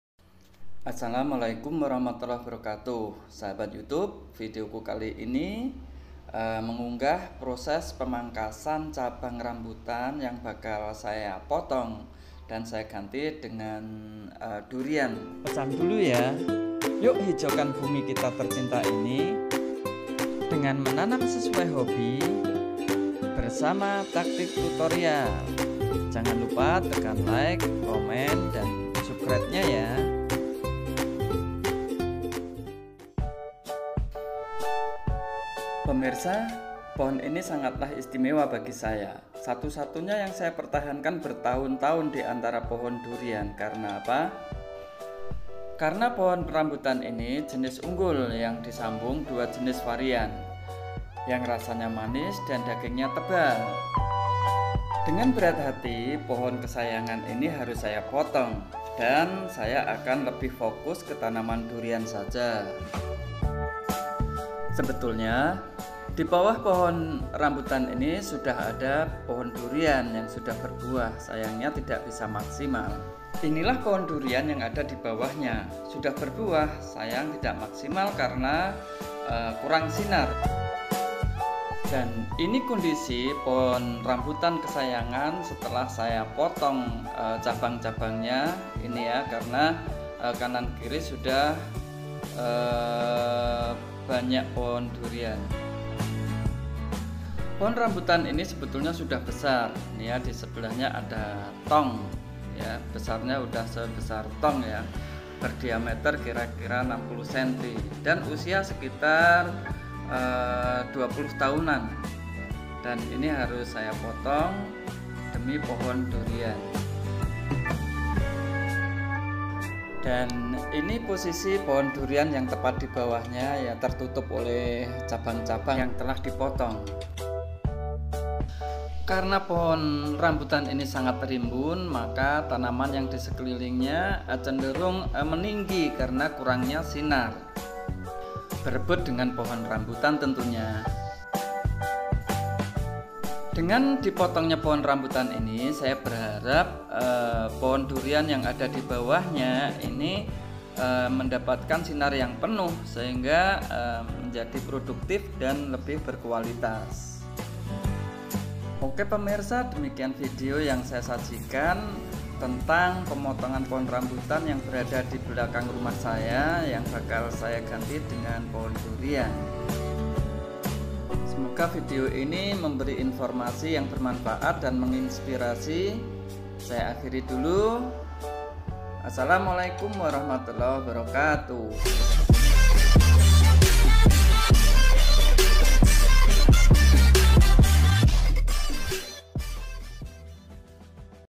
Cara agar kebun durian produktif potong pohon rambutan kesayangan
Cara agar kebun durian produktif potong pohon rambutan kesayangan. sebetulnya pohon durian dan rambutan bisa hidup berdampingan dengan baik sayangnya karena lahan terbatas dan daun rindang pada rambutan membuat pohon durian sedikit kurang sinar. akhirnya satu satunya pohon rambutan koleksi kesayangan harus rela dipotong demi kualitas dan produktifitas pohon durian di sekelilingnya.